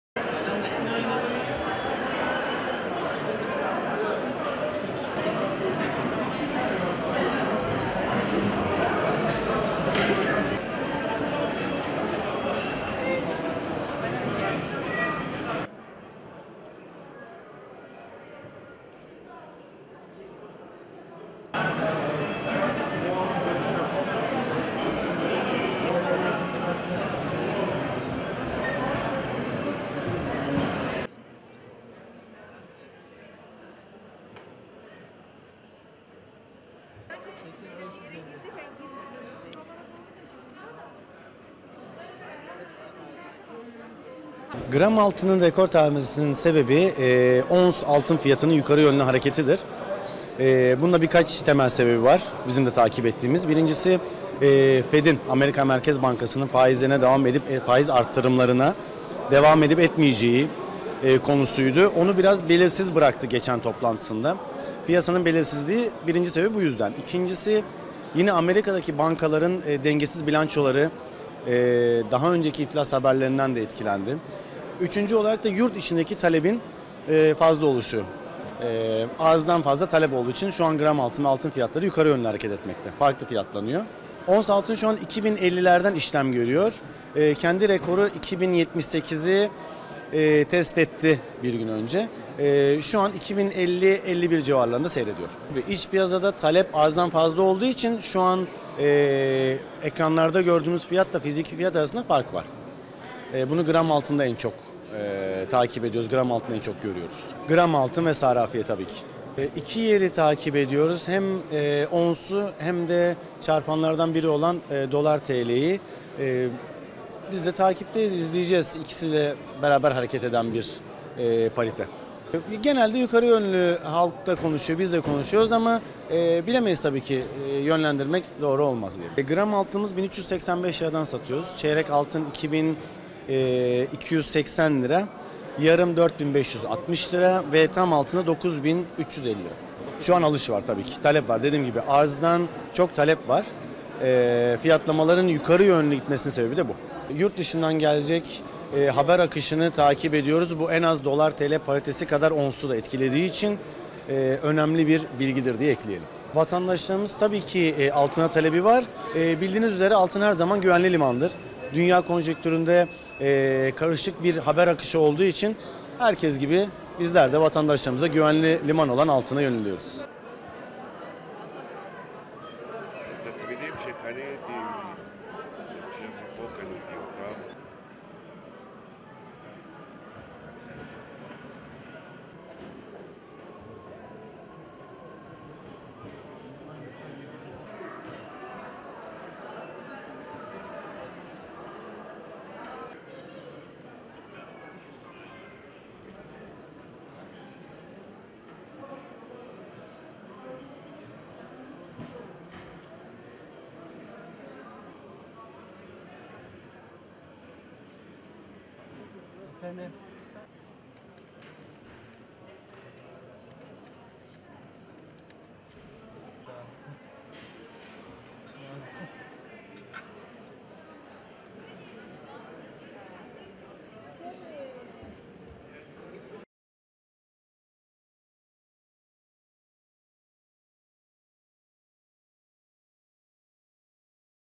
Altında yeni rekor, gram 1385 liradan işlem görüyor
Gram altın rekor tazelemeye devam ediyor. Kapalıçarşı’da gram altının satış fiyatı 1385 lirayı gördü.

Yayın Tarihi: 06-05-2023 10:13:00